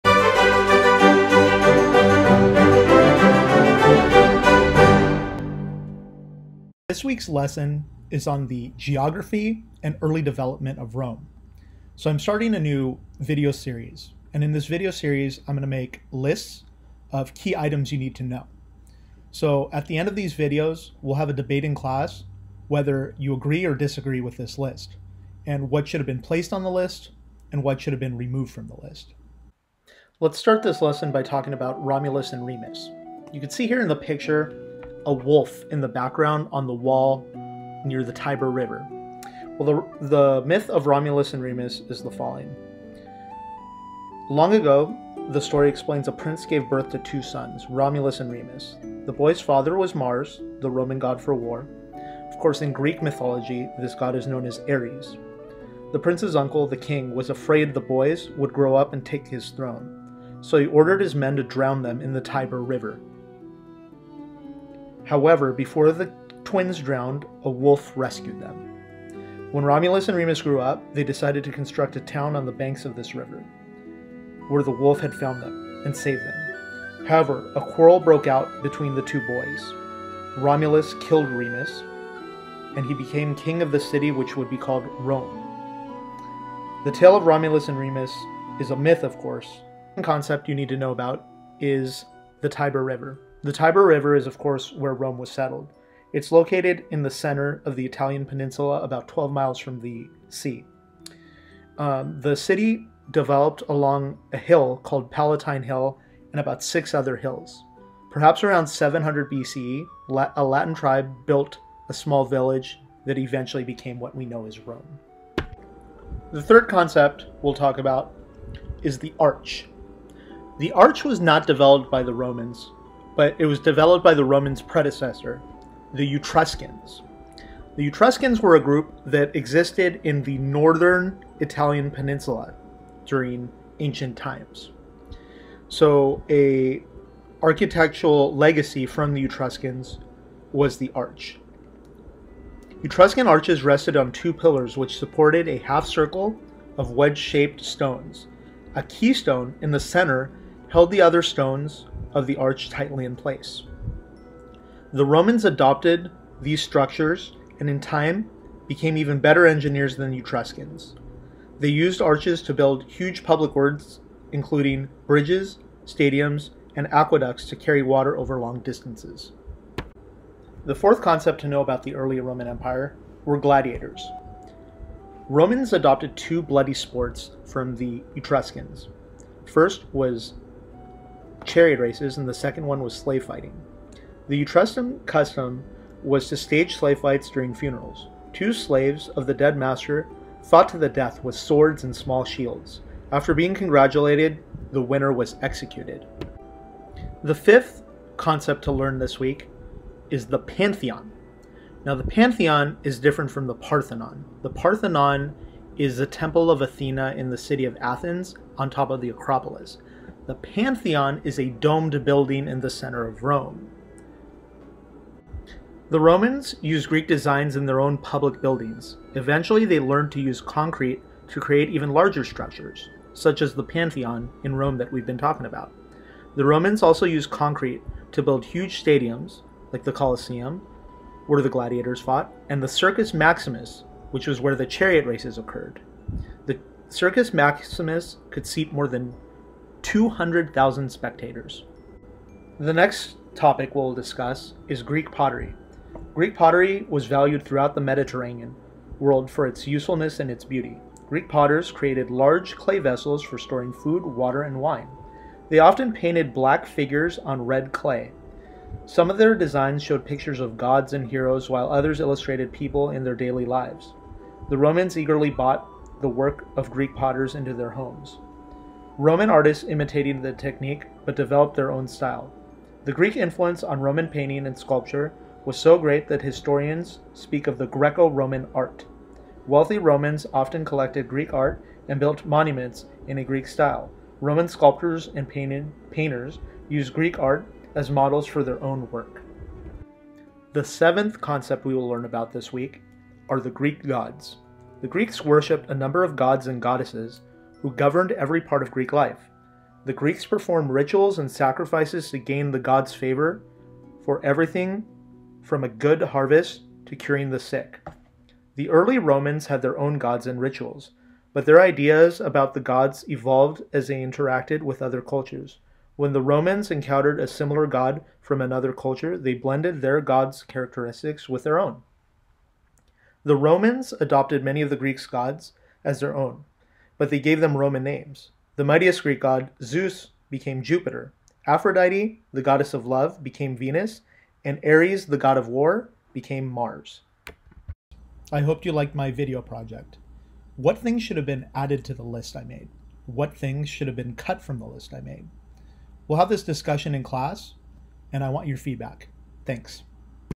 The Greeks and the Etruscans: Roman Influencers
Pepes rambles about Early Rome and the legacy of the Greeks and the Etruscans. What should have been added to the list? What should have been left out?

Topics
1. Romulus and Remus
2. Tiber River
3. Arch
4. Gladiators
5. The Pantheon
6. Greek Pottery
7. Greek Gods